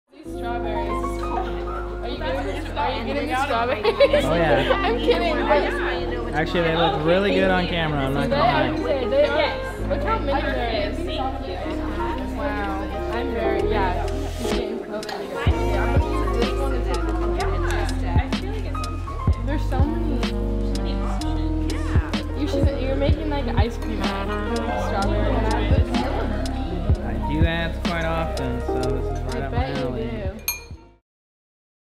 it's just ice cream, strawberries, and shortcake for dessert // Sony A7 III
Shot this on my Sony A7 III back in May 2020 while visiting some friends and family in Palm Springs for my Dad's birthday. The video was only really intended to be published on someone's Instagram page, but after finding it again while organizing my old footage and projects...

It's kind of a vibe.

I mean, just look at all that ice cream and strawberries and shortcake... put that dessert lounge music on in the background and it's game over.

So anyway, I've decided to publish it on my channel despite the unusual 1080 by 1080 square aspect ratio.

Austin Ziprick // Ziprick Arts & Enterprises (ZAE)